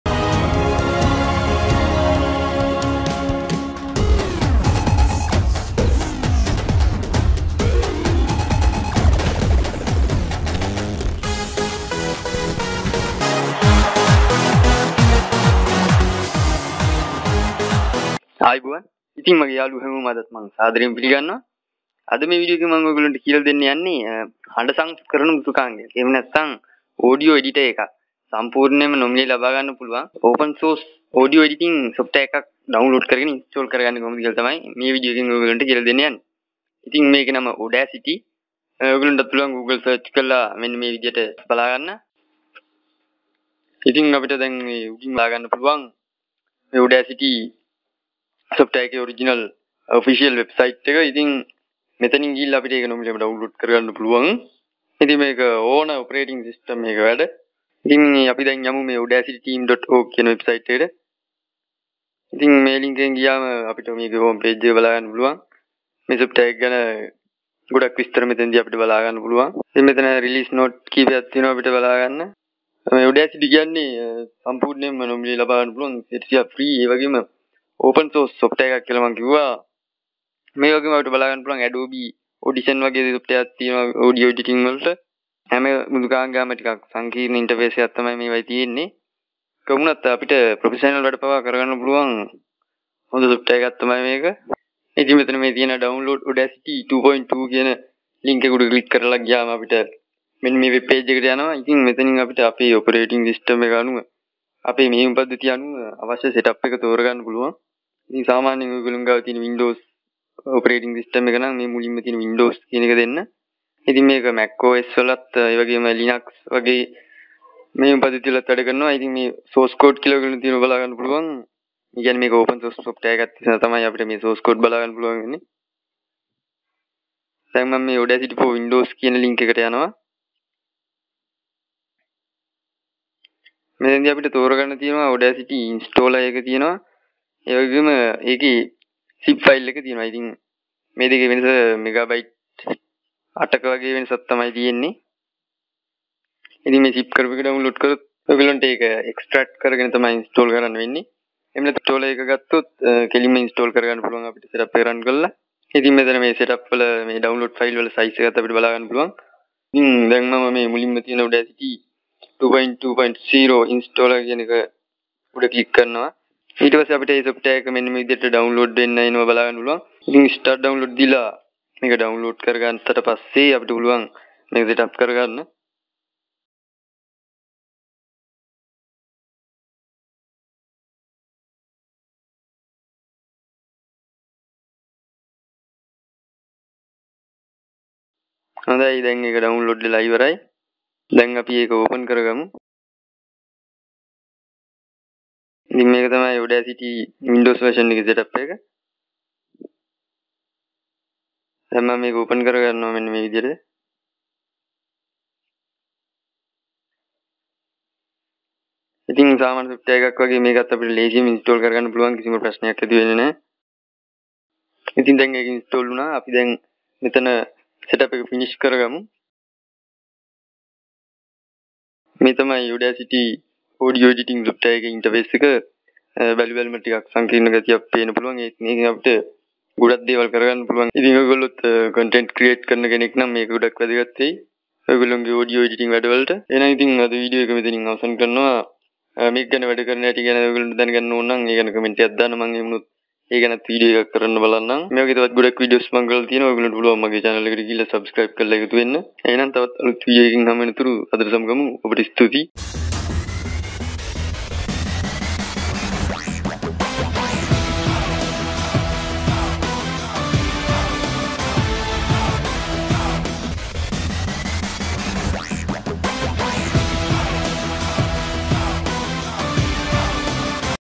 Audacity Free Audio Editor - How to Download & Install
Download & Install Audacity Free Audio Editor properly and starting to edit audios sounds voice anyway you need... This is totally free and open source 100% working software. Best one for normal content creators & video editors ( vloggers ). You can do many advanced things in this field with this application successfully.. (source code available). Some basic things have explained clearly in sinhala . Link is simple... :) This program mostly using for remove noise (noise reduction) of a video, change pitch.. depth, echo effect, voice corrections,.. etc.

To Save as MP3 in high quality you should have lame_enc.dll file in your pc computer. If you don't have it You can download it from Internet.